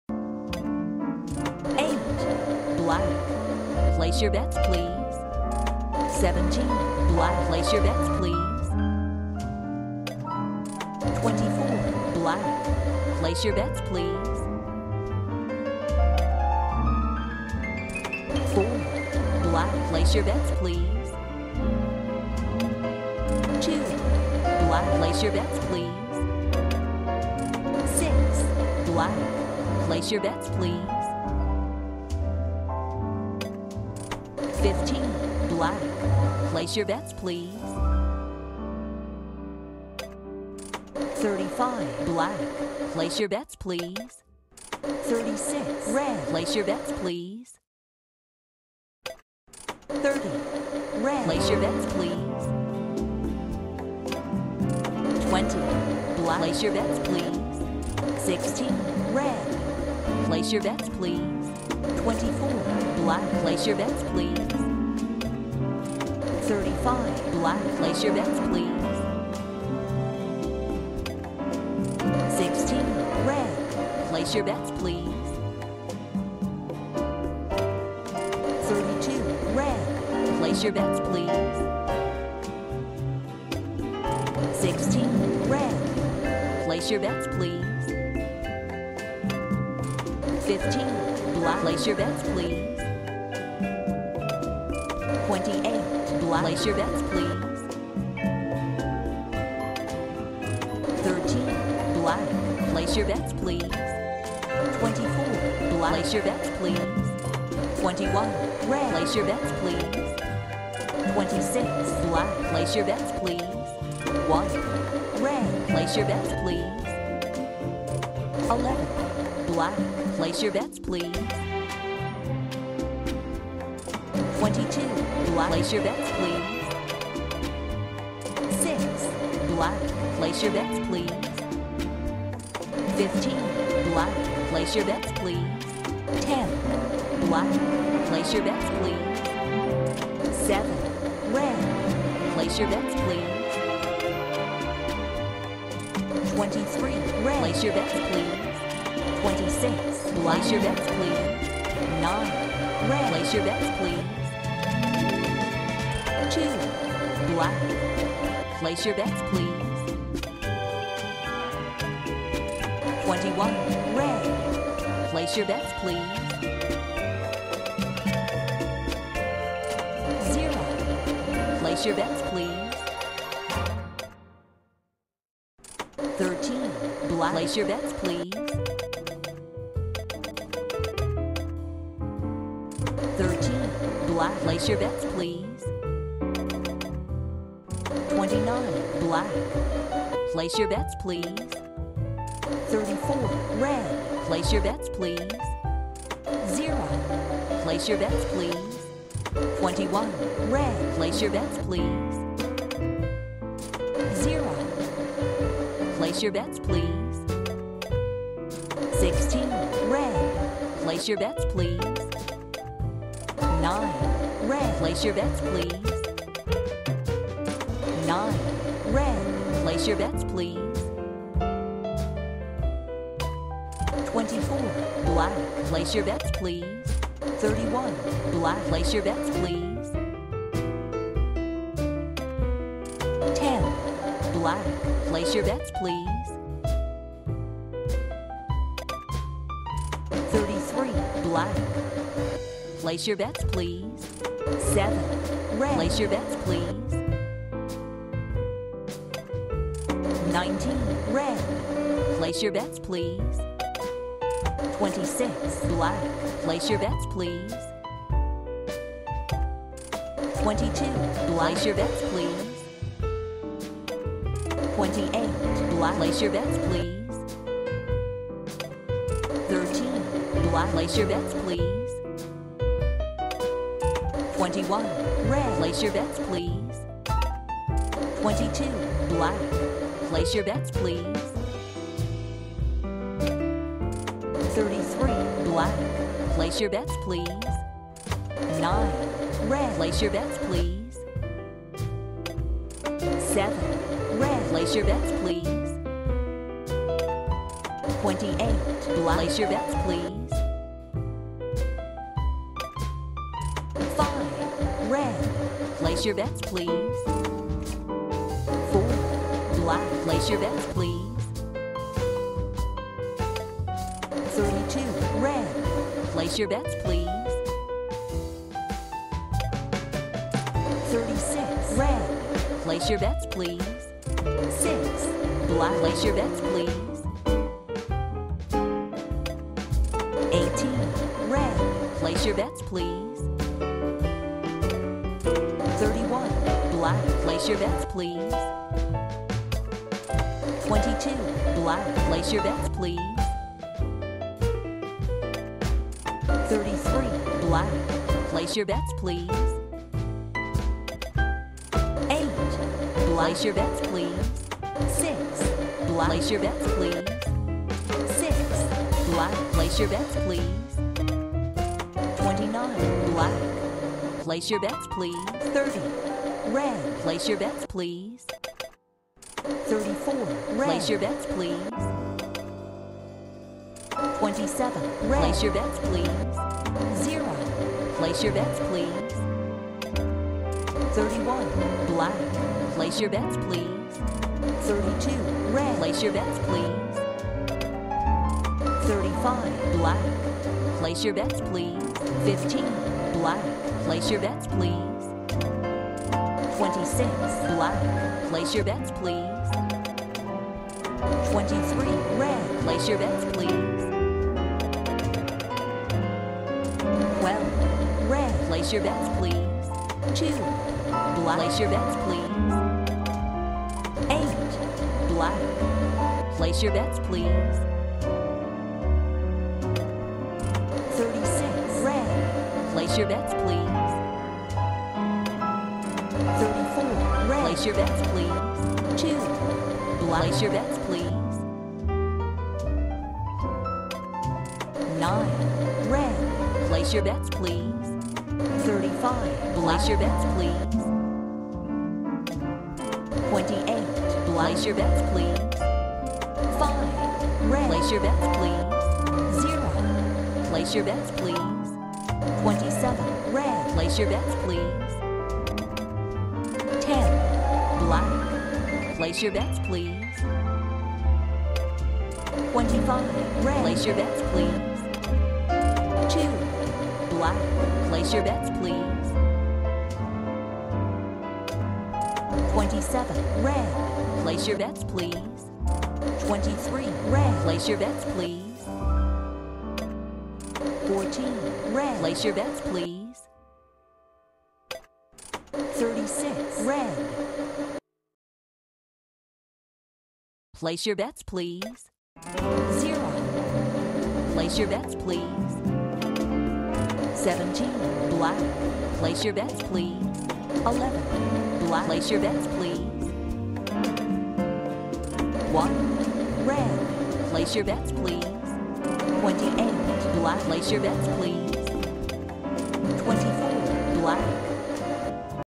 Roulette Software - Real Money - €31,300 - €31,700
Tremendous session, where I make another €400 and take my balance up to €31,700.